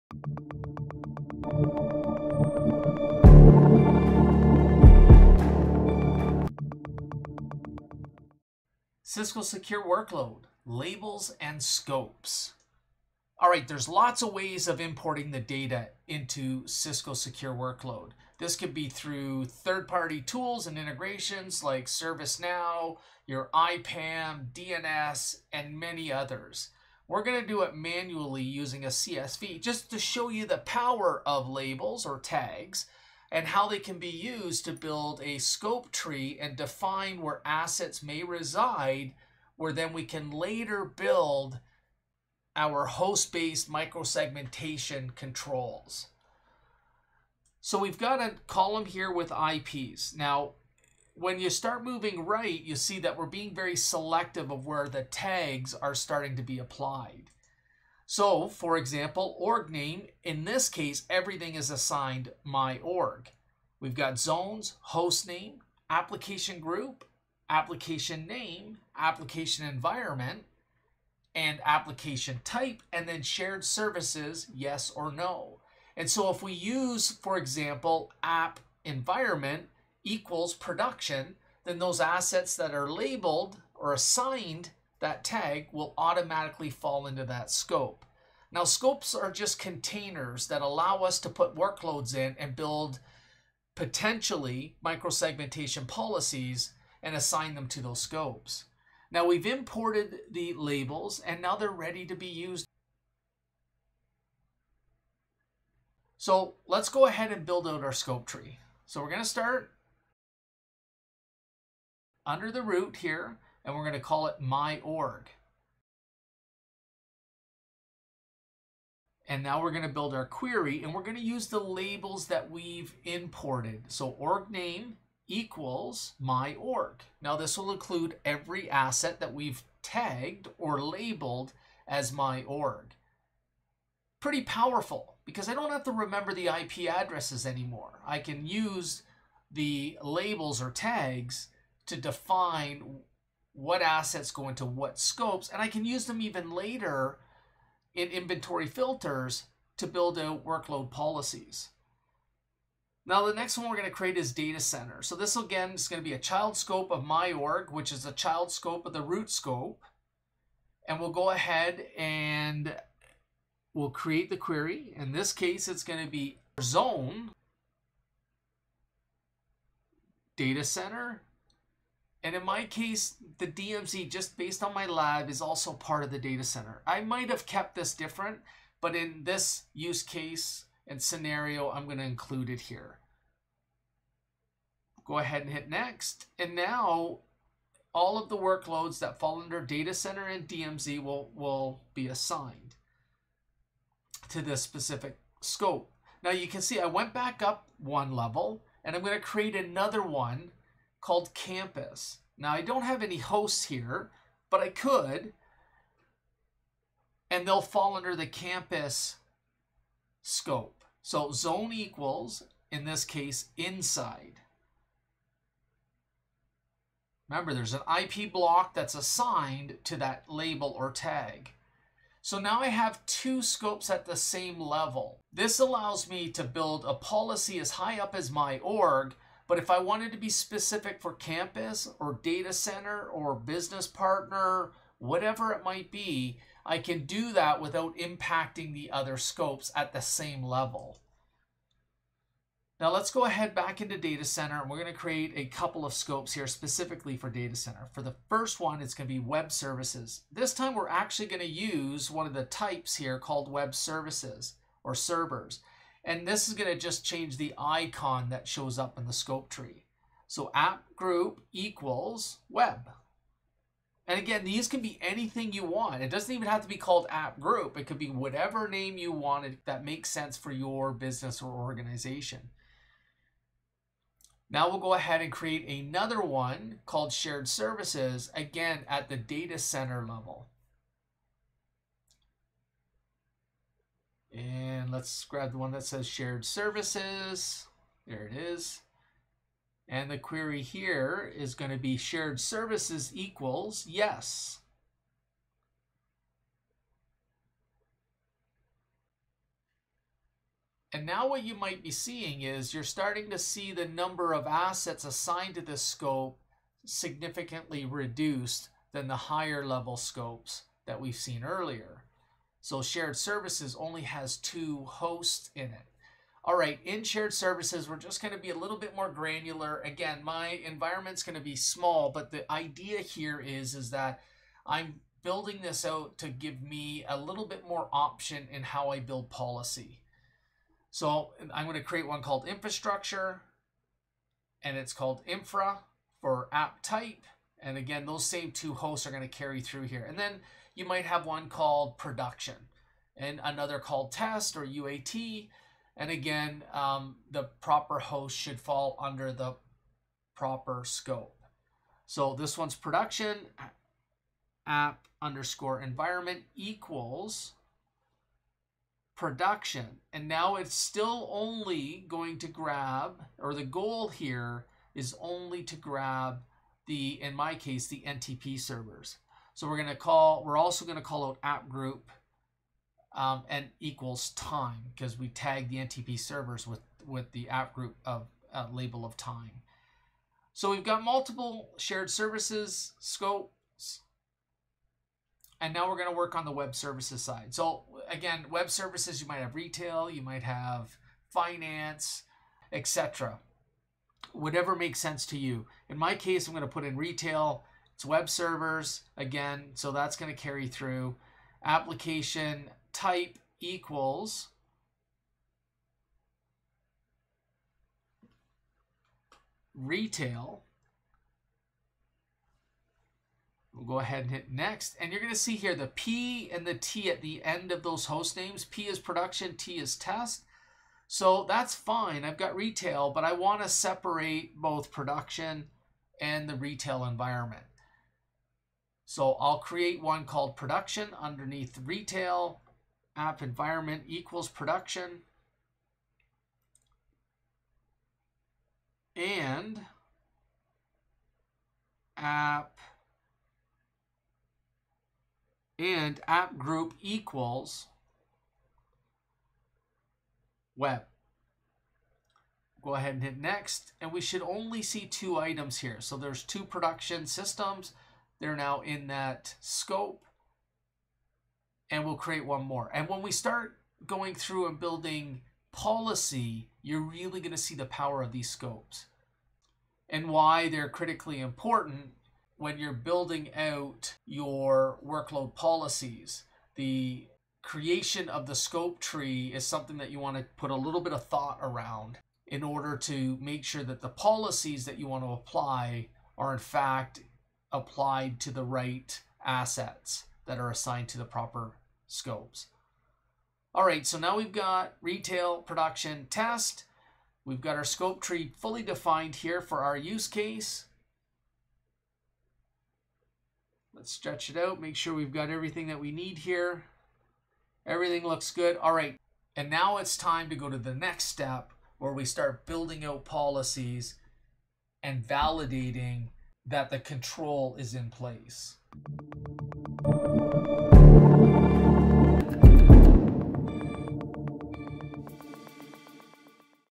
Cisco Secure Workload: Labels and Scopes:
In this video we will walk though labels, importing labels manually, and then building out a scope tree using the labels and have to automatically assigned to their respective scopes. This will allow us to assign workspaces to build out microsegmentation policies.